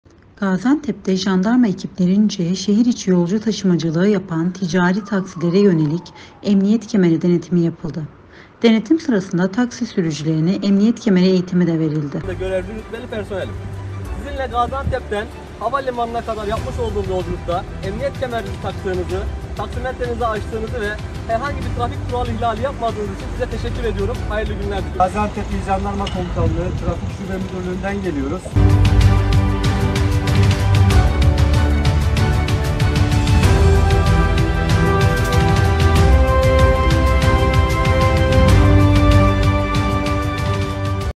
Ticari Taksilerde Emniyet Kemeri Denetimi
Gaziantep'te jandarma ekiplerince şehir içi yolcu taşımacılığı yapan ticari taksilere yönelik emniyet kemeri denetimi yapıldı. Denetim sırasında taksi sürücülerine emniyet kemeri eğitimi de verildi.